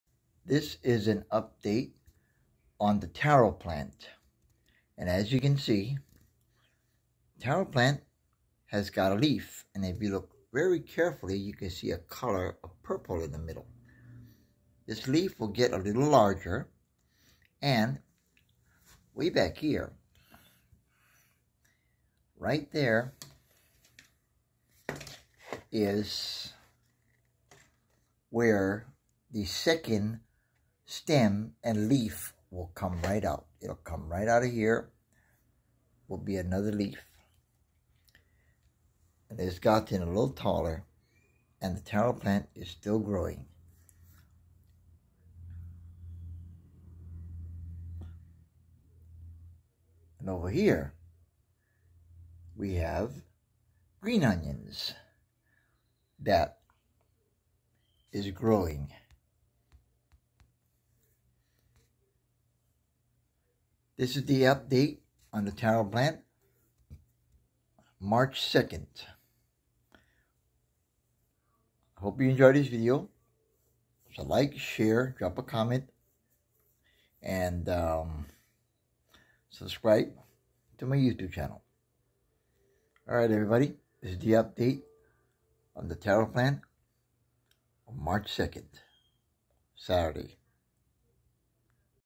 taro plant gotten BIGGER update5
this taro plant has a leaf and a second stem about to
grow from the first one that will branch out and have more LEAVES.